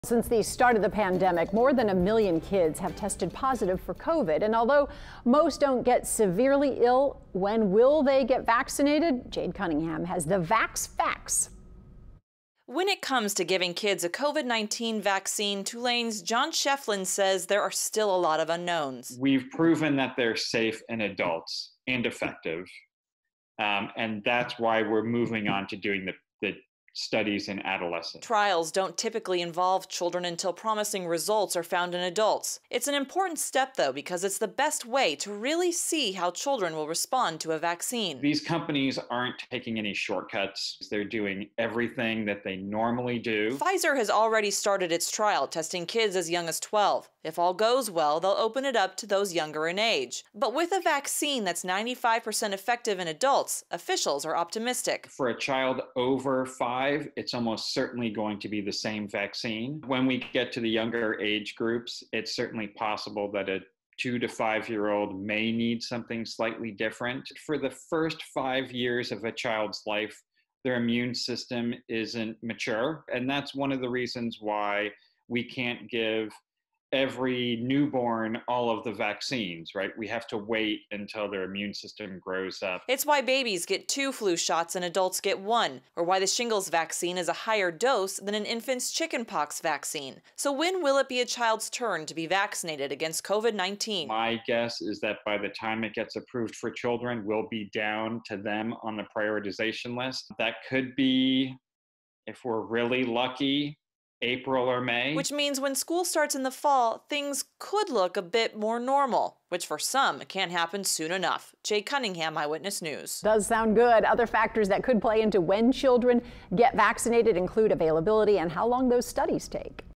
Vax facts about COVID-19 vaccine and kids
With the high number of covid cases in children many are asking when will kids be able to get the vaccine.